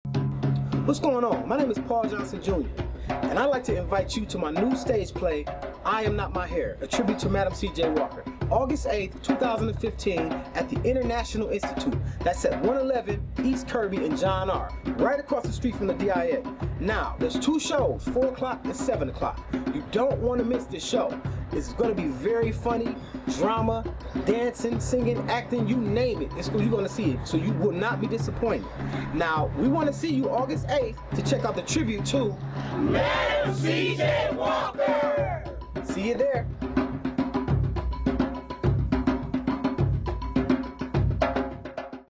I Am Not My Hair - Promo Commercial - Stage Play by Paul Johnson Jr.
313 Soul Entertainment, LLC brings you "I am Not my Hair". Written and Produced by Paul Johnson Jr. Co-Directed by Rodney D. Story. Coming to the International Institute (111 E. Kirby at John R.) Saturday, August 8, 2015. TWO SHOWS 4pm and 7pm.